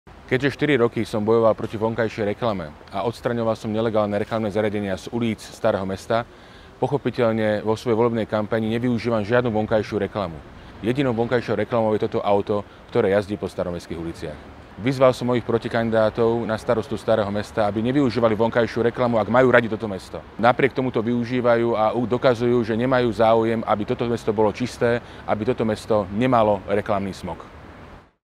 VOLEBNÁ KAMPAŇ STAROSTU BEZ BILBORDOV
Keďže štyri roky bojujem s vonkajšou reklamou v uliciach Starého Mesta, odstránili sme desiatky bilbordov a citylightov, 49 nelegálnych reklamných búdok a zo staromestských chodníkov sme vytlačili 34 reklamných trojnožiek, rozhodol som sa nevyužívať vo volebnej kampani vonkajšiu reklamu. Na staromestských uliciach uvidíte len toto auto, bojujem proti reklamnému smogu a nebudem ho na rozdiel od mojich protikandidátov využívať.